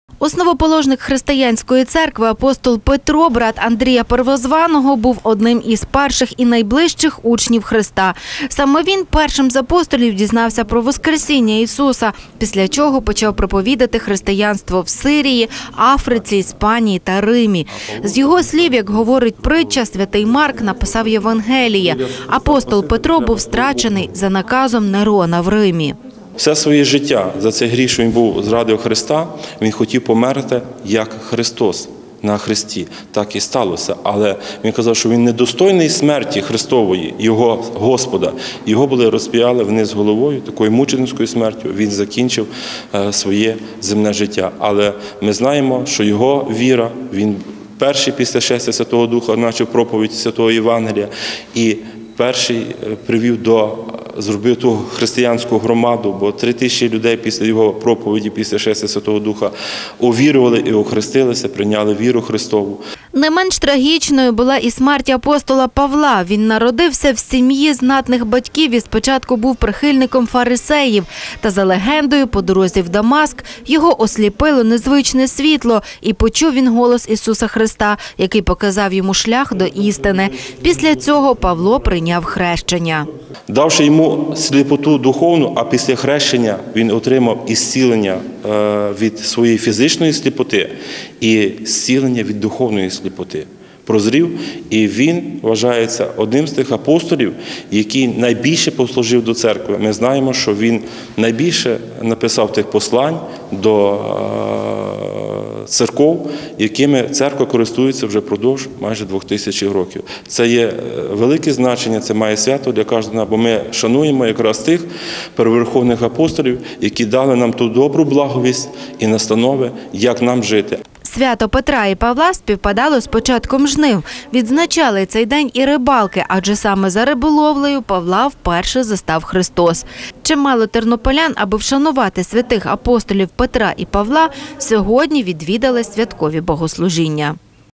12 липня - Петра і Павла
Сьогодні християни відзначають День Святих апостолів Петра і Павла. Сьогодні ж - закінчується Петрівський піст. За традицією, цього дня слід відвідувати рідних та друзів, пригощати їх рибними стравами. До свята господині завжди прибирали садибу, пiдбiлювали та пiдфарбовували хату, вiшали чистi рушники.